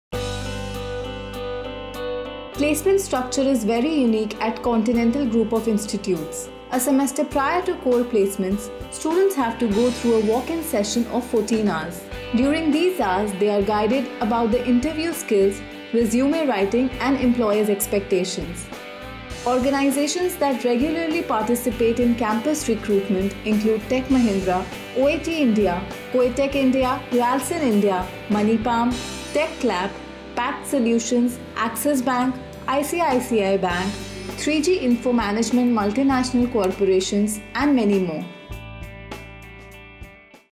Continental Group of Institutes Placement Updates
Placement structure is very unique at Cotinental Group of Institutes. A semester prior to core placements students have to go through a walk in session of 14 hrs. During these hours they are guided about the interview skills, resume writing and employers expectations.
Organizations that regularly participate in campus recruitment include Tech Mahindra, OAT India, Coatec India, Ralson India, Moneypalm, TechClap, PACT Solutions, AXIS and ICICI Bank, 3G Info Management multinational corporations.